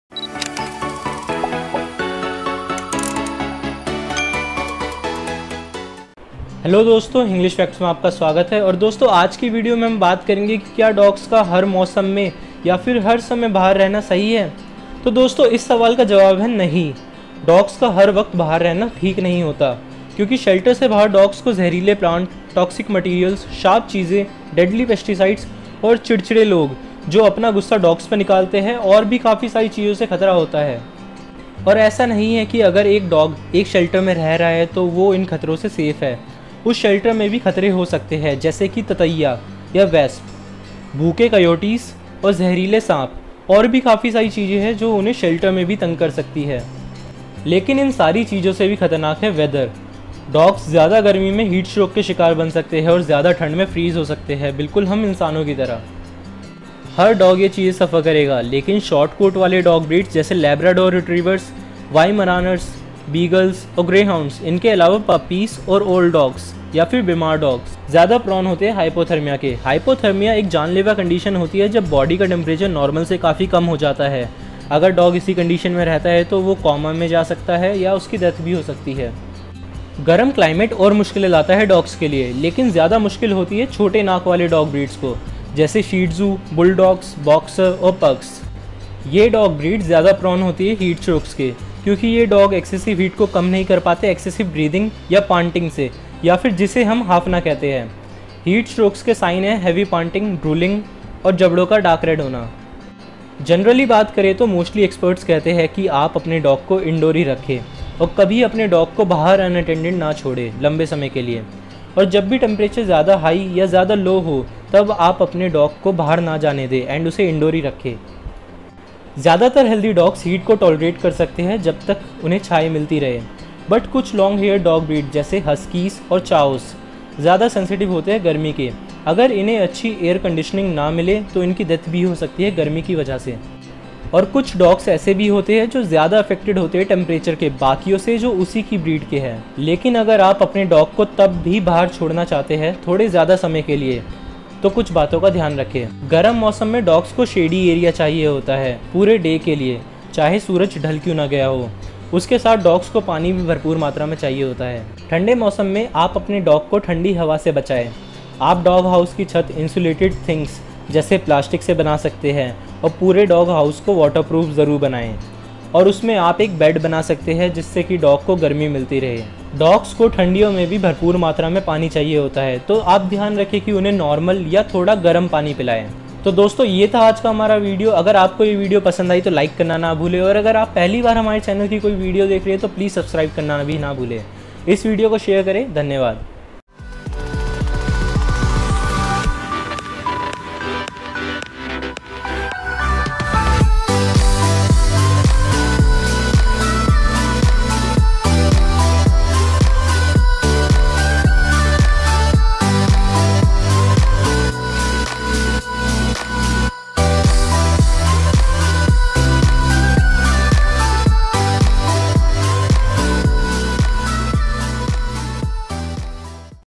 Can Dogs Live Outside In All Season ? | DOGGY GYAN
The backyard dog is fast becoming a thing of the past. Decades ago, it was common for dogs to live their entire lives outside in suburban and urban yards. But as our knowledge of canines has evolved, we've learned that staying outside 24/7 can be hazardous for a dog's health.
The list of potential dangers is long and varied: poisonous plants, puddles of toxic antifreeze, sharp gardening tools, deadly pesticides, cruel teenagers and revengeful neighbors -- just to name a few. Even when confined to a fenced-in yard, dogs aren't safe from other wildlife that can still easily enter. Think yellow jackets, poisonous snakes and hungry coyotes.
Any dog will suffer if left outside in extremely low temperatures, but shorthaired breeds like Labrador retrievers, Weimaraners, beagles and greyhounds, as well as young, old or ill dogs are most susceptible to hypothermia, a potentially deadly condition where body temperature falls below normal. Signs include shivering, lethargy and lack of coordination. As the condition progresses, dogs may become comatose and die.
Warmer climates present other challenges for canines, especially for short-nosed (brachycephalic) breeds like shih tzus, bulldogs, boxers and pugs. These, along with other brachycephalic breeds, are more susceptible to heat stroke because of their inability to effectively rid their bodies of excess heat by panting. Geriatric pets are vulnerable to overheating as well. Signs of heat stroke include excessive panting, drooling and gums turning deep red. If left untreated, overheated pets may perish.

and more on HINGLISH FACTS
LOVE DOGS.